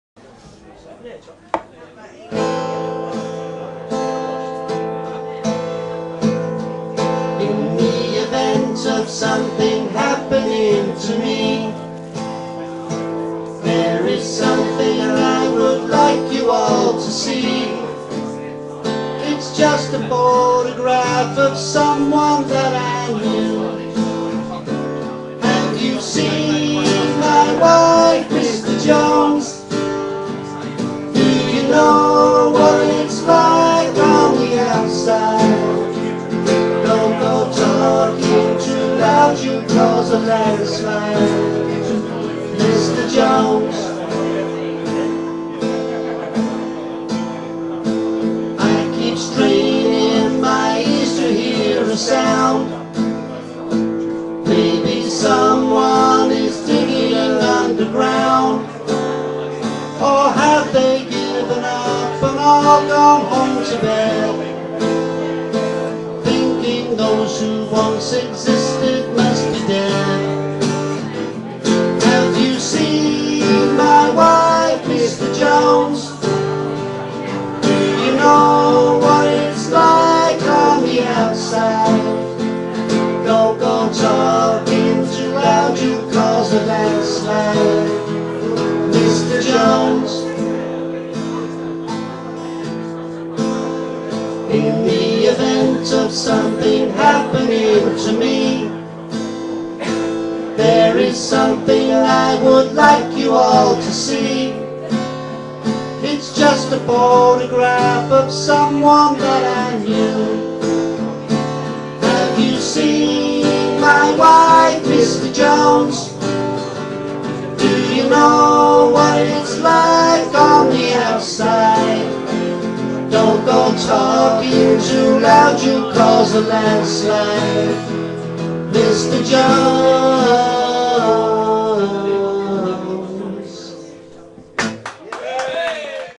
Bob & Lee 2
Mando's Open Mic night at The Miley,Rochford Essex.26th July 2012